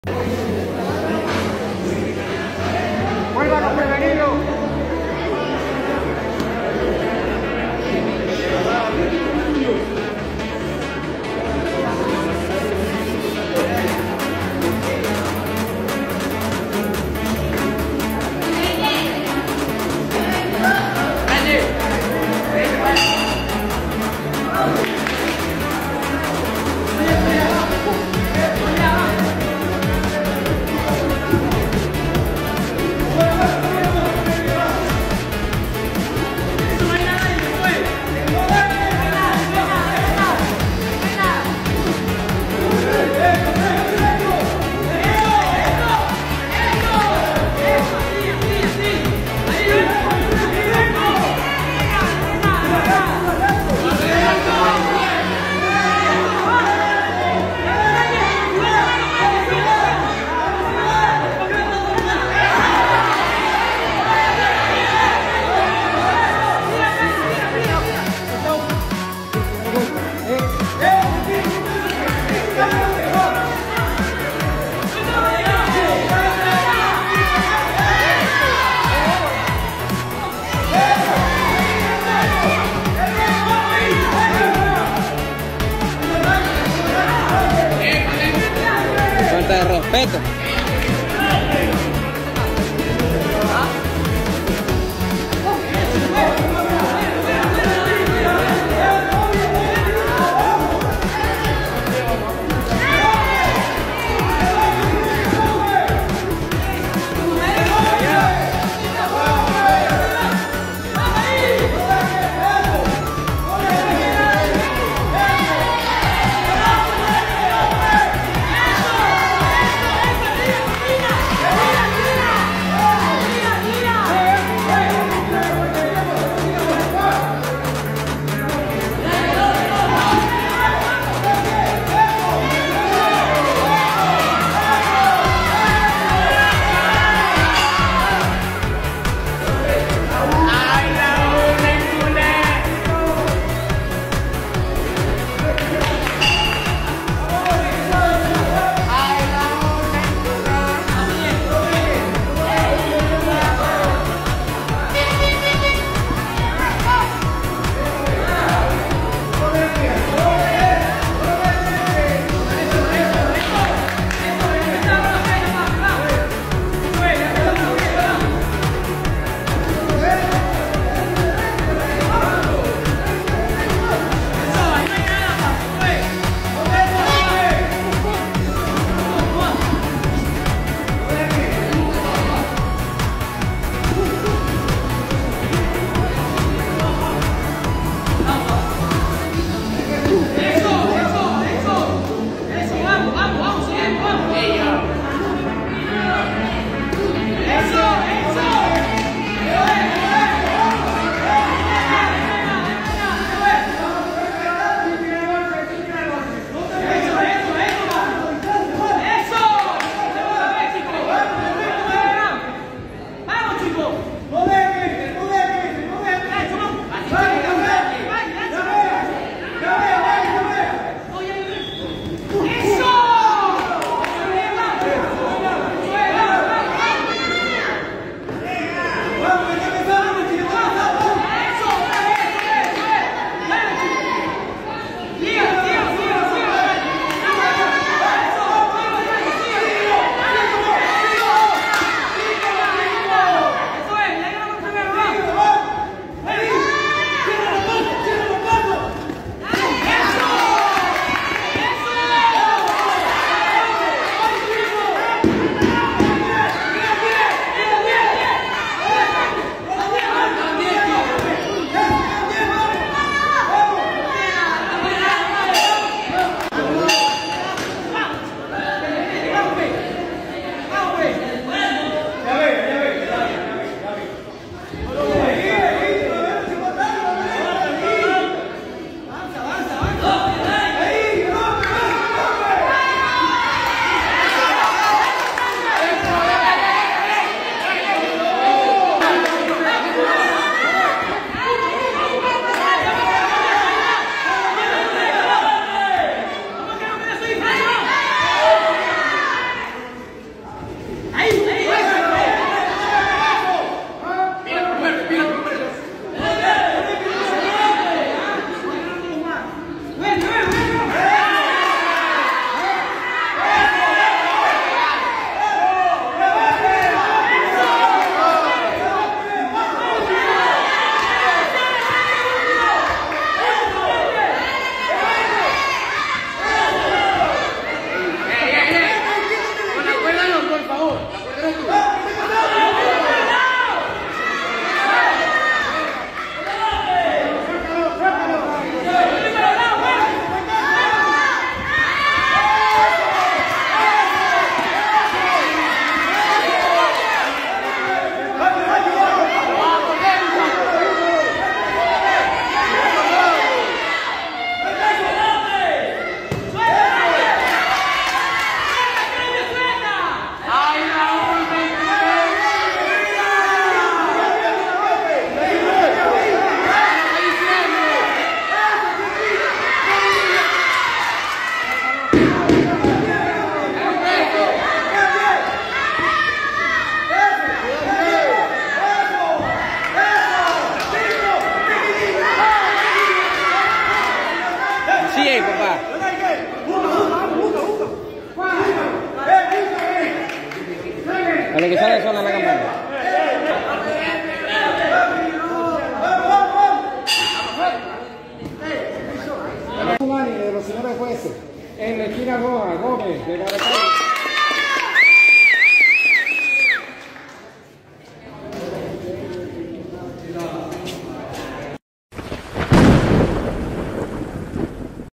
Escuela de Boxeo Johnner Gomez 🥊🥊 Coliseo de Boxeo Pedro Gamarro la Cotorrera 🥊🥊
Gimnasio Don José Beracasa Maracaibo
Combate del Atleta Johneiker Gomez ( Roja ) 🆚 Franyer Hernández ( Azul ) dónde Gomez se título ganador por margen 3-0 ante Hernández.🥊🥊🥇🇻🇪